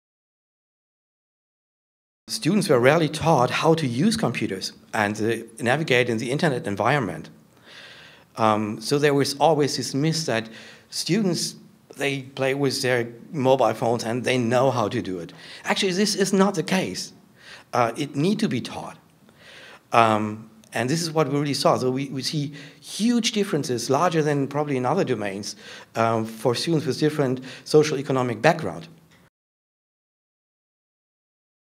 Thinking Beyond Borders: The Future of Student Assessment (Excerpt of Dirk Hastedt)
Dirk Hastedt, an expert on the TIMSS and PIRLs assessments, tells Governing Board members that it's a myth that students can understand computers because of their personal use of technology.